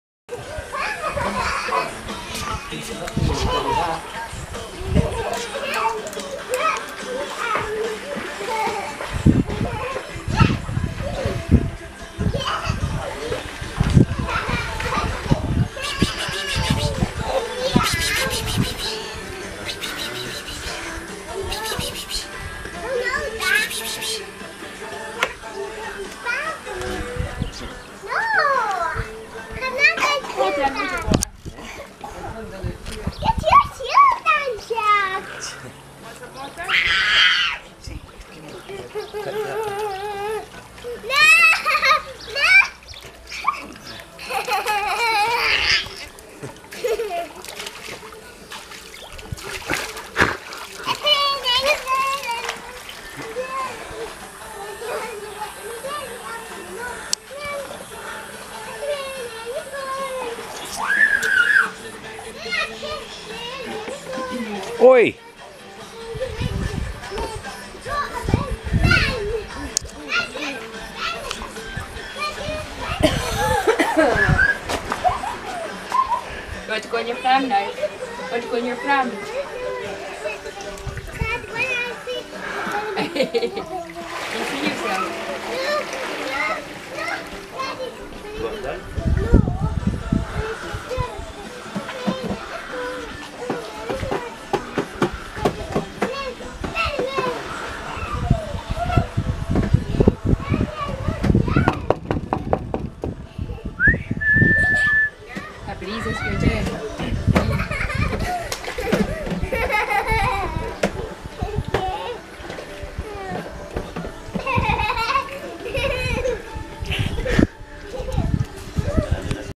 Gail Paul Georgia,Jockson :) at the Pool North cyprus (hoilday)
Georgia laughing Jaxon eating, maw wearing the same dress lol and happy parents enjoying them self's